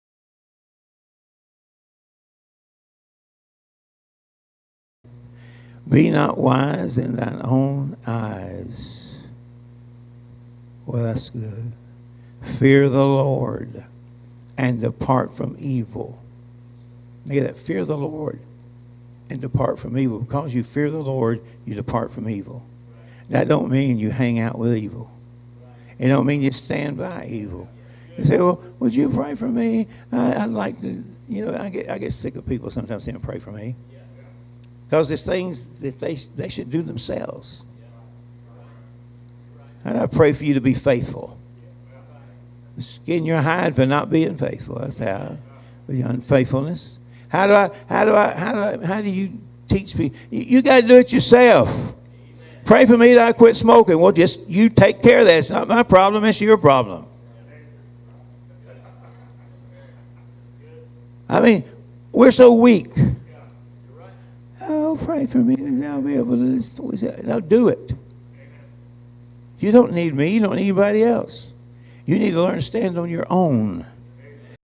Tom Neal is Tired of People Asking Him to Pray for Them
In the sermon If Foundations Be Destroyed, preached in 2020 at Grace Baptist Church of Eufaula, AL, Tom Neal says he gets tired sometimes of people asking for prayer, that his way of praying for them is to skin them for not doing what they should be doing.

As a man who was a pastor, I'm sure Tom Neal has dealt with a number of people with differing problems and issues. I'm sure he's dealt with people who struggle with the "weight and sin that doth so easily beset us." I'm sure that as a man, it may be frustrating to deal with certain things or deal with the same things over and over from some people.

But as a pastor, that is part of shepherding the sheep. That is part of the responsibility that God gives to a pastor. And to say that their problems are not your problems is to ignore what the scripture says.

The Bible says in James 5:16, "Confess your faults one to another, and pray one for another, that ye may be healed. The effectual fervent prayer of a righteous man availeth much."

In I John 5 we read, "And this is the confidence that we have in him, that, if we ask any thing according to his will, he heareth us: And if we know that he hear us, whatsoever we ask, we know that we have the petitions that we desired of him. If any man see his brother sin a sin which is not unto death, he shall ask, and he shall give him life for them that sin not unto death. There is a sin unto death: I do not say that he shall pray for it." In other words, we are to pray for our brethren when we see them sin so that God can work in their lives.

But it appears that Tom Neal would rather just "skin you," because that does seem to be an easier course of action that praying and teaching.